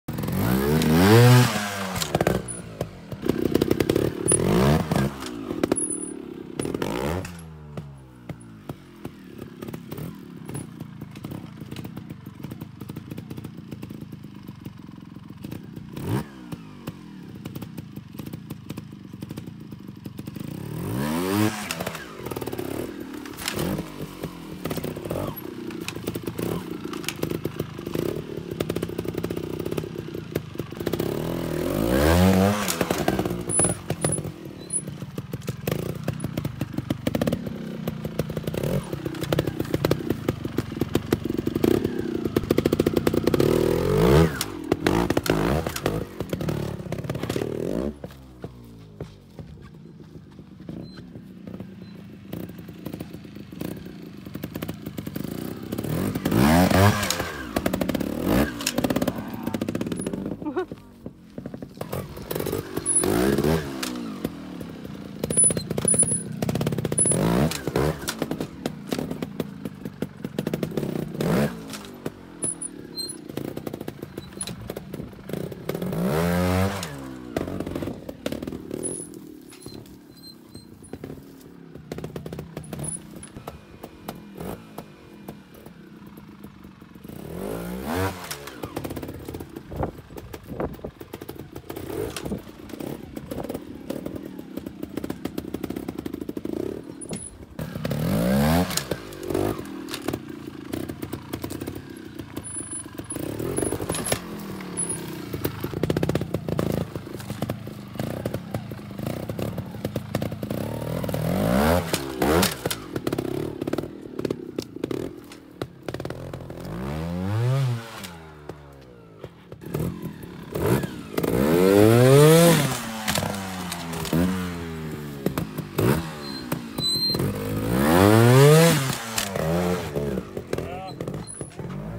Louise Forsley California Trials Training
Getting some training in prior to the the Texas Super Trial and Trials Nationals this summer.